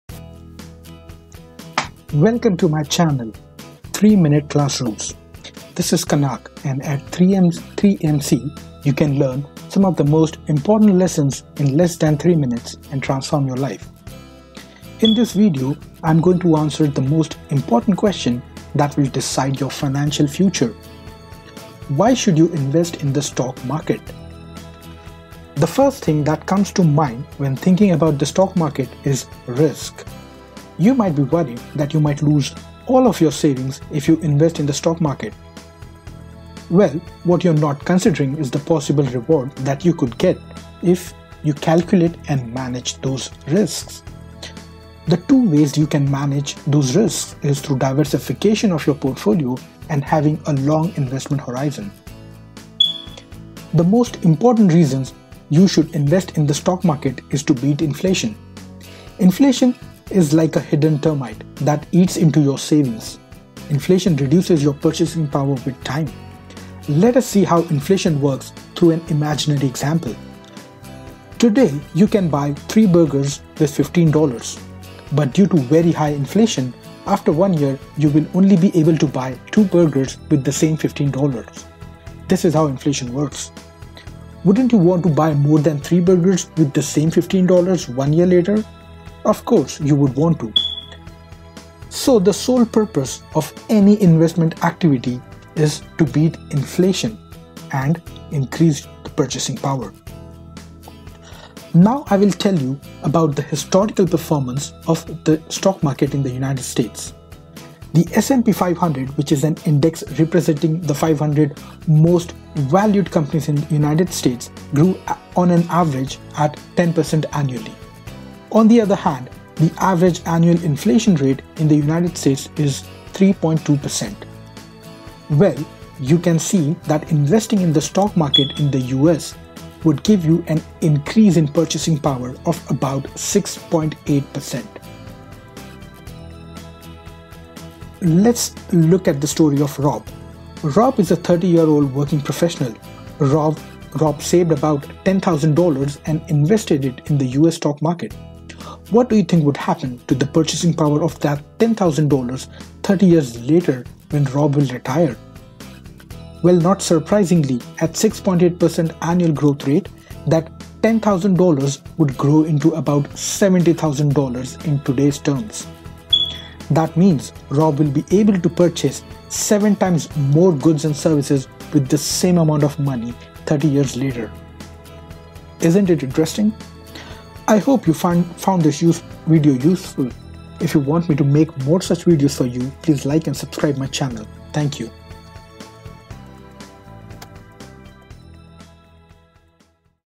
Stock Market Investing: The One Reason You Should Start Now
If you're looking to grow your wealth, investing in the stock market could be the key. There are countless reasons to invest in the stock market, but there's one that stands out above the rest: the potential for long-term growth.

When you invest in the stock market, you're essentially buying ownership in a company. As the company grows and becomes more profitable, the value of your investment increases. And if you hold onto your investments for the long-term, you could see significant growth over time.

Of course, there are risks involved in investing in the stock market. The value of your investments can go up or down depending on the performance of the companies you invest in. But historically, the stock market has provided solid returns over the long-term.

Another reason to invest in the stock market is the power of compounding. When you reinvest your earnings and dividends back into your investments, your money grows exponentially over time.

And finally, investing in the stock market is a way to hedge against inflation. As prices for goods and services rise, the value of your money decreases. But by investing in the stock market, you have the potential to earn returns that outpace inflation, preserving the value of your money over time.

In summary, there are countless reasons to invest in the stock market. But if you're looking for just one reason, it's the potential for long-term growth. By investing in the stock market, you have the opportunity to build wealth over time and achieve your financial goals.